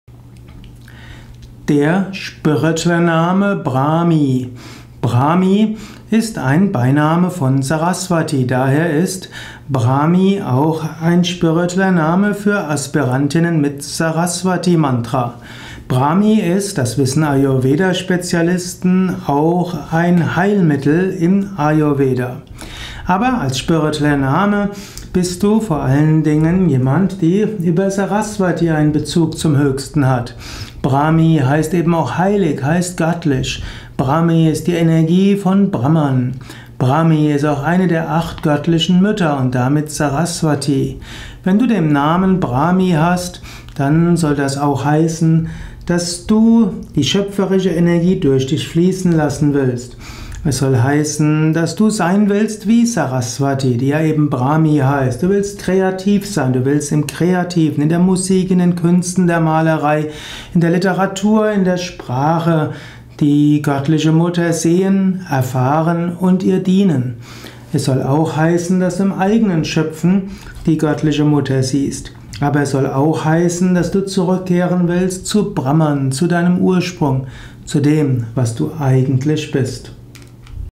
Spiritueller Name Brahmi - Bedeutung und Übersetzung aus dem Sanskrit
Was ist die Bedeutung von Brahmi als Sanskrit Name, als indischer Name, als spiritueller Name? Brahmi die Energie von Brahma; die göttliche Mutter aller Wesen; die Göttin der Sprache; die Göttin allen Werdens; Beiname von Saraswati. Brahmi ist ein indischer Name. Wenn du also jemanden aus Indien kennst, der/die diesen Namen hat, dann kannst du hier erfahren, was dieser Name bedeuten kann .Dies ist auch ein Namen, der im Westen Yoga Aspirantinnen als spiritueller Name gegeben werden kann. Brahmi wird insbesondere vergeben an Aspirantinnen mit Saraswati Mantra. Wenn du Brahmi als spirituellen Namen hast, bekommst du ein paar Anregungen, was dieser Name für dich bedeuten könnte.

Hier in Kürze die Zusammenfassung: Brahmi, Sanskrit ब्राह्मी brāhmī f., die Energie von Brahma; die göttliche Mutter aller Wesen; die Göttin der Sprache; die Göttin allen Werdens; Beiname von Saraswati.